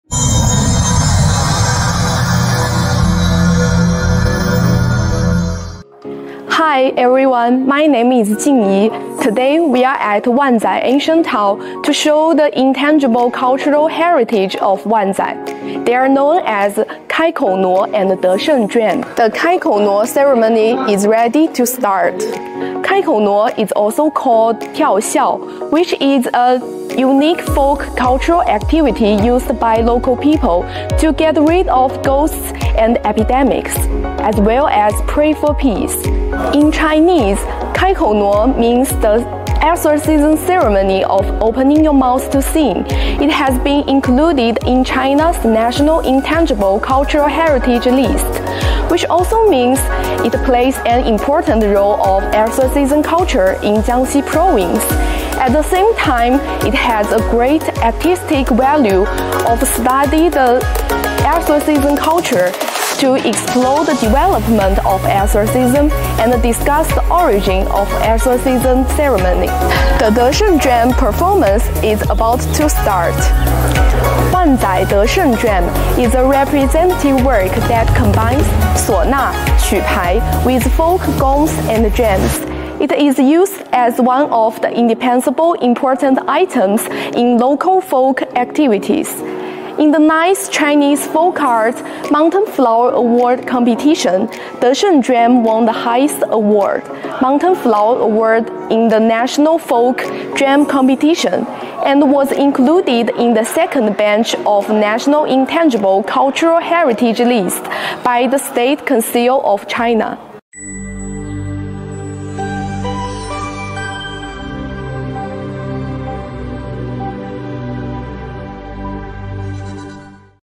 Wanzai Ancient Town, the home of Wanzai intangible cultural heritage
Today, we are going to visit the Wanzai Ancient Town to learn about the intangible cultural heritage of Wanzai.